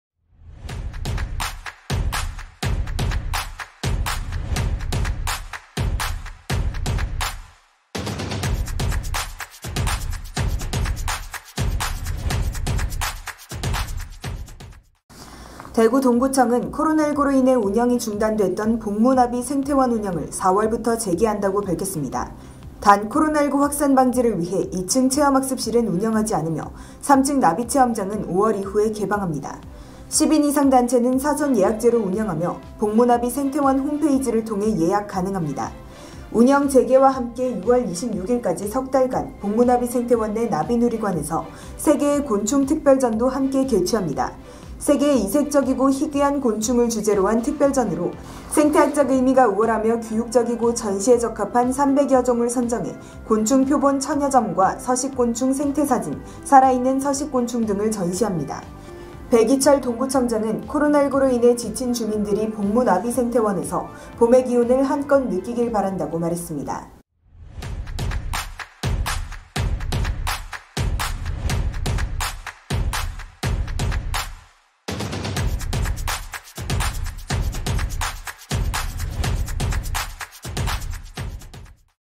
대구 동구청 봄무나비생태원 4월 개장 경북도민방송TV
대구 동구청은 코로나19로 인해 운영이 중단됐던 봉무나비생태원 운영을 4월부터 재개한다 밝혔습니다.

단, 코로나19 확산 방지를 위해 2층 체험학습실은 운영하지 않으며, 3층 나비체험장은 5월 이후에 개방합니다.

10인 이상 단체는 사전예약제로 운영하며, 봉무나비생태원 홈페이지를 통해 예약 가능합니다.

운영 재개와 함께 6월26일까지 석 달 간 봉무나비생태원 내 나비누리관에서 ‘세계의 곤충 특별전’도 함께 개최합니다.

세계의 이색적이고 희귀한 곤충을 주제로 한 특별전으로, 생태학적 의미가 우월하며 교육적이고 전시에 적합한 300여 종을 선정해 곤충표본 1천여 점과 서식곤충 생태사진, 살아있는 서식 곤충 등을 전시합니다.

배기철 동구청장은 “코로나19로 인해 지친 주민들이 봉무나비생태원에서 봄의 기운을 한껏 느끼길 바란다”고 말했습니다.

손중모 기자